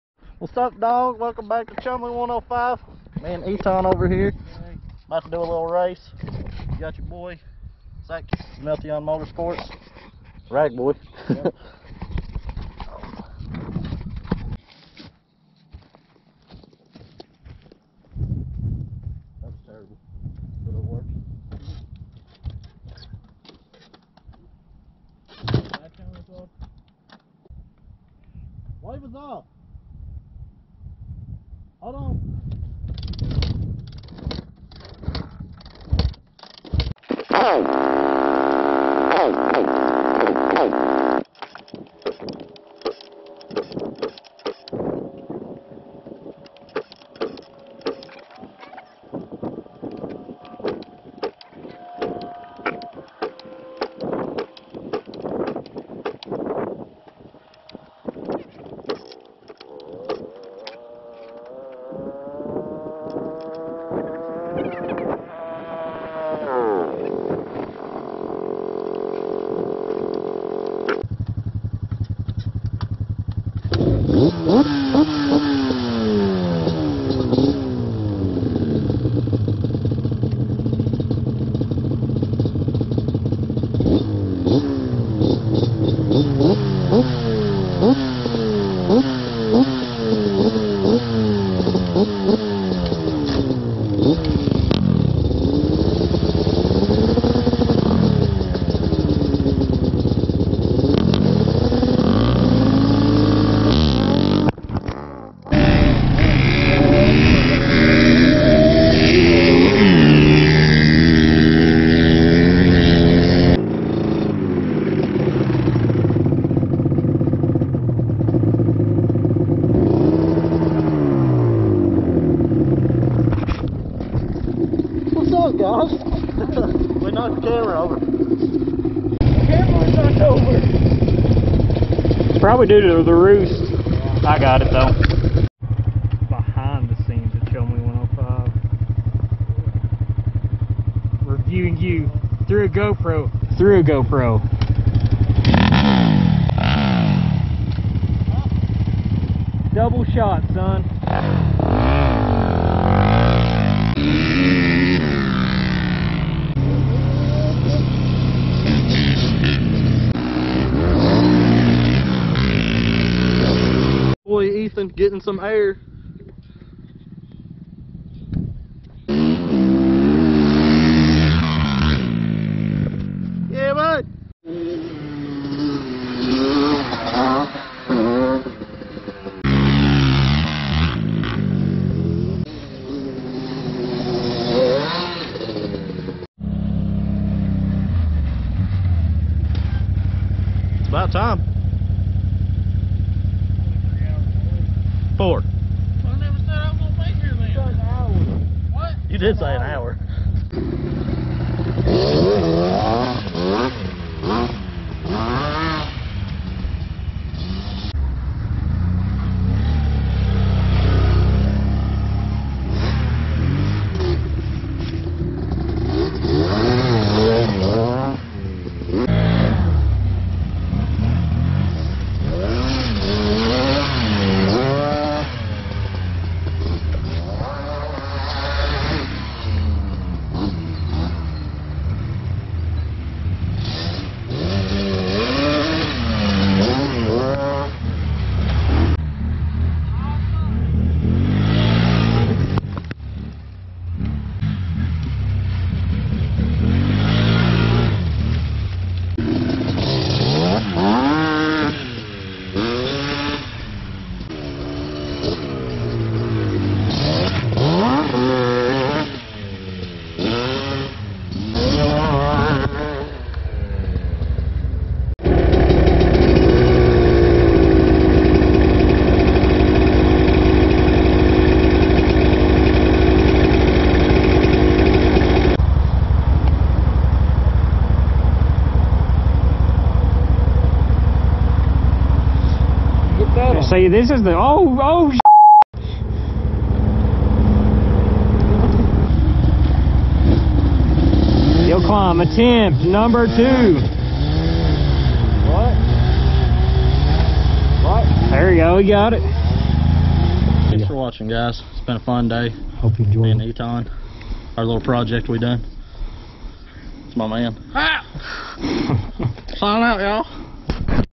Playing in the Dirt! (Yz250f/Banshee/Renegade)
Slinging some dirt on this episode of chumley105! Hope you guys enjoy.